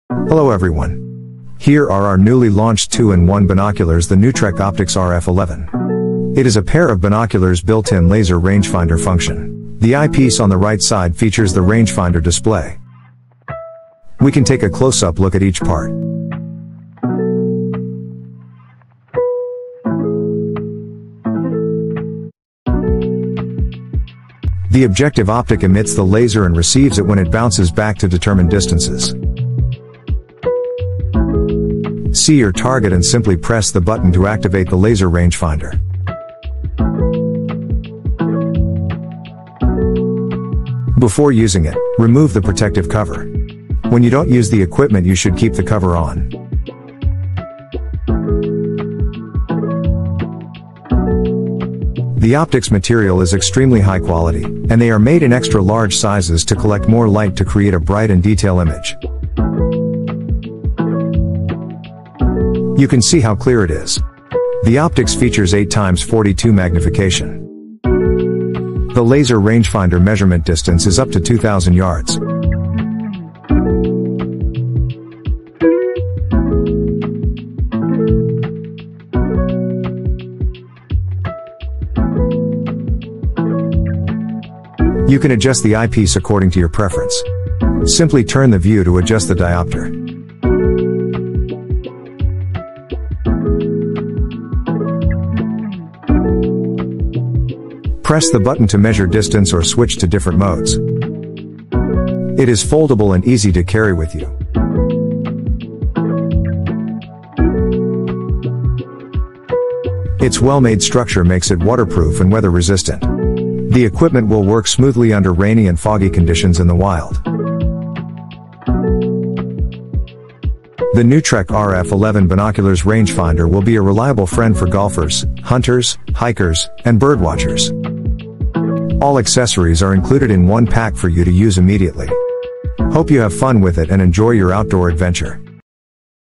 Nutrek Optics Binoculars Rangefinder(RF11)
Nutrek Optics RF11 Binoculars Rangefinder-The Nutrek RF11B Series is two products in one – binocular and rangefinder – making it a perfect instrument for outdoor activities. Multi-layer anti-reflection coating provides a contrast-rich, sharp, and brilliant image even in difficult lighting conditions.